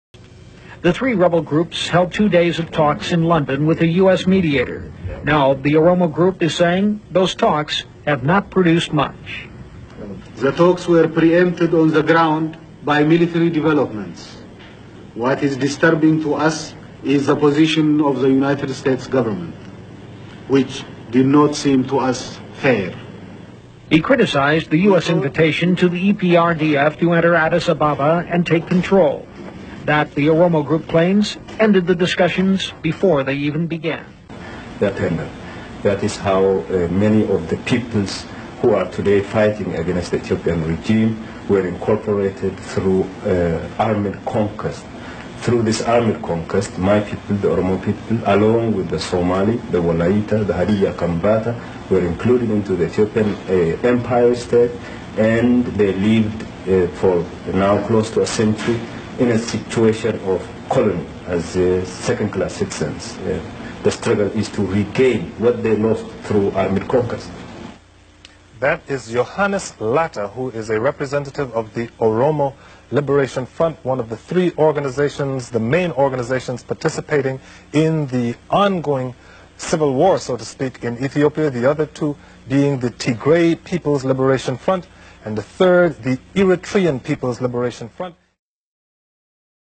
Lencho Leta 1991 during Transitional government when He was OLF leader
With Dima Negawo